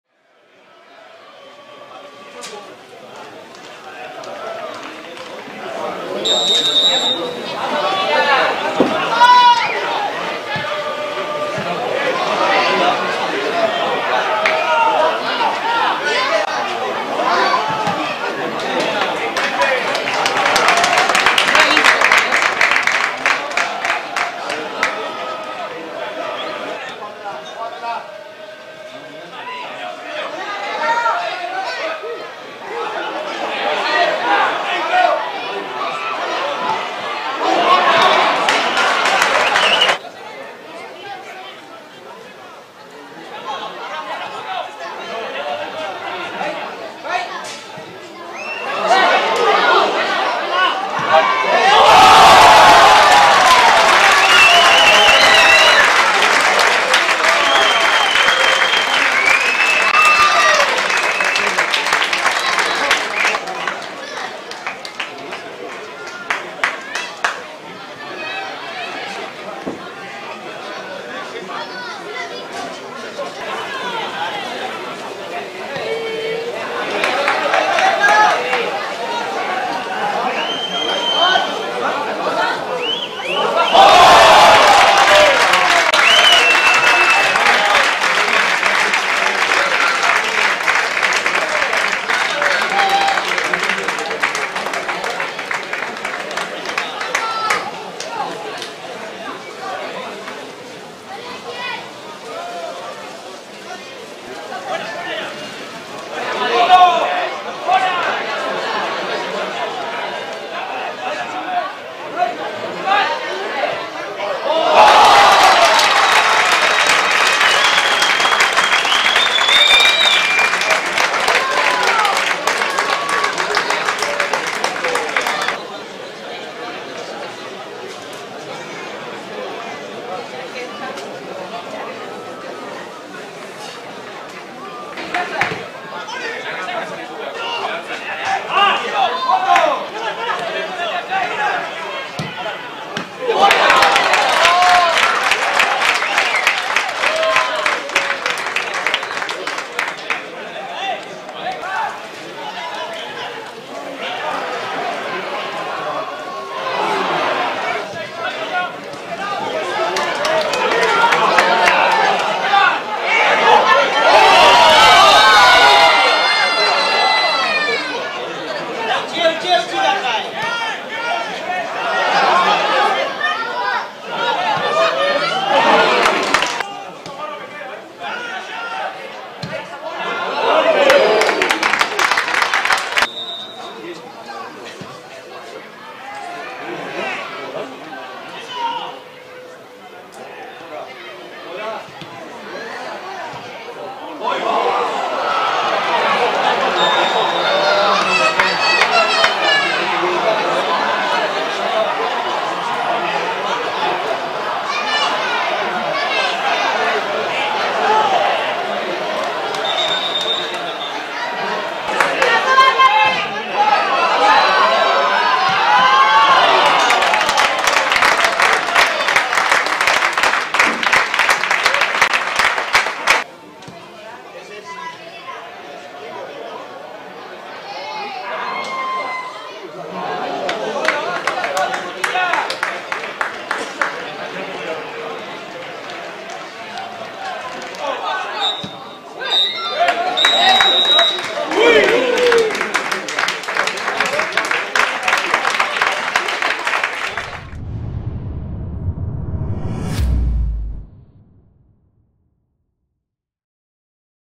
RESUMEN PrimeraFederación | Real Sociedad de Fútbol B 3-0 SD Logroñés | Grupo 2 | Jornada 18
⚽ 1-0 | Maguna (21')
⚽ 2-0 | Javi Martón (27')
⚽ 3-0 | Jorge Agirre (41')

📍 Zubieta